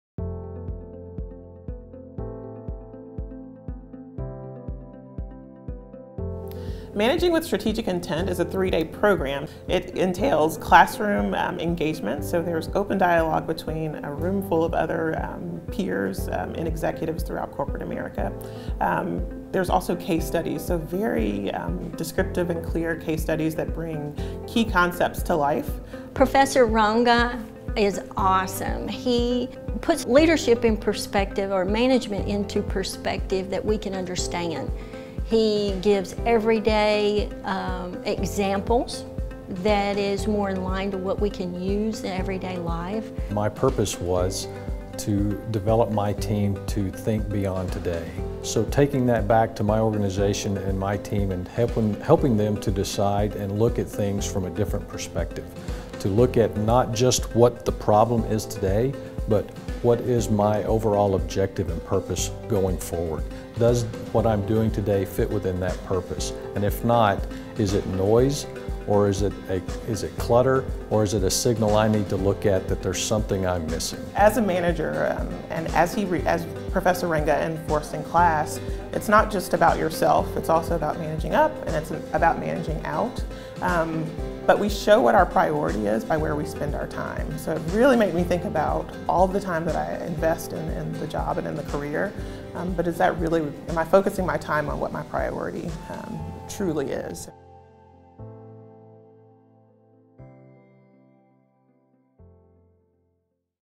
Managing with Strategic Intent
Vanderbilt Executive Education offers Managing with Strategic Intent: a 3-day program offered regularly on Vanderbilt’s campus. Through a combination of case studies, discussion, lecture and reflection, you’ll improve your ability to manage employees and achieve strategically driven results.